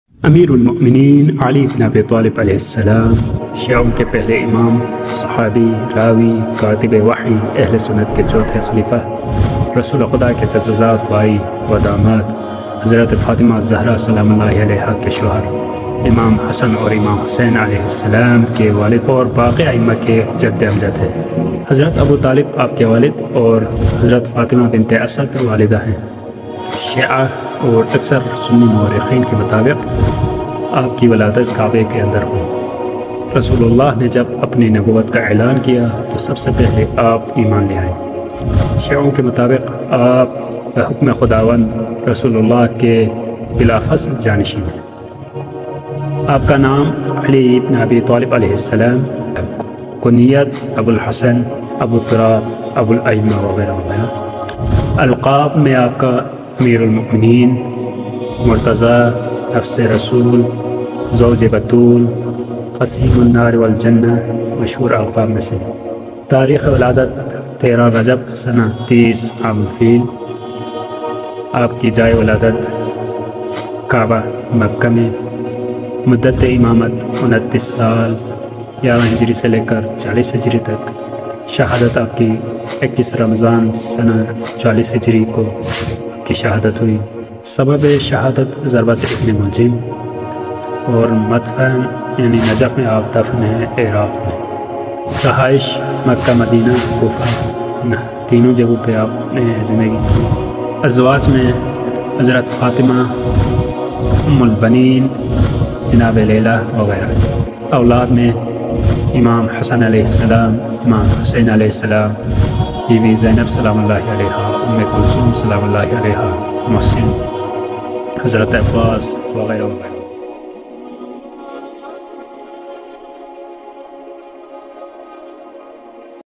The Untold Story of Ali, Mola Ali in Urdu
In this video, we delve into the life and legacy of Mola Ali, a revered figure in Islamic history known for his bravery, wisdom, and devotion to God. From his birth to his marriage to the Prophet Muhammad's daughter, Fatimah, to his leadership during battles, Mola Ali's story is one of courage, sacrifice, and spirituality. Join us as we explore the teachings and wisdom imparted by this iconic figure and discover why he is known as the Lion of God.
nahnu shia ali ibn abi talib

Mola ali Muslim karbala shia islam امیرالمومنین علی حضرت علی